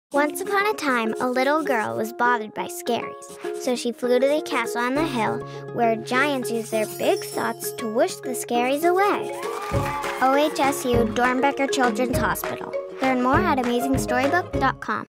Amazing Storybook
Once upon a time, a little girl was bothered by scaries, so she flew to the castle on a hill.

Learn more – or share your Doernbecher story here: AmazingStorybook.com